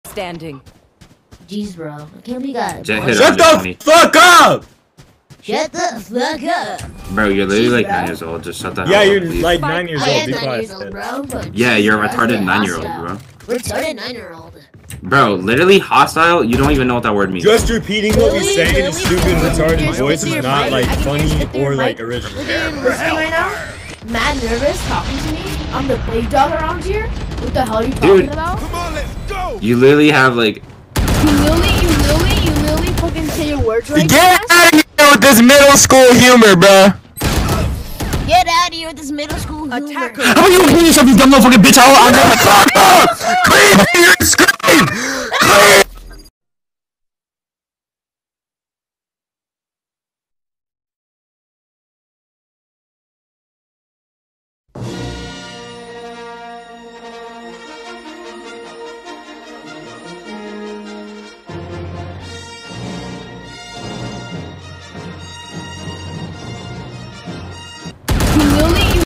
Kid Gets Yelled At On Valorant || Animation
You literally, You literally, You literally, You literally
They got too angry over Mahnon...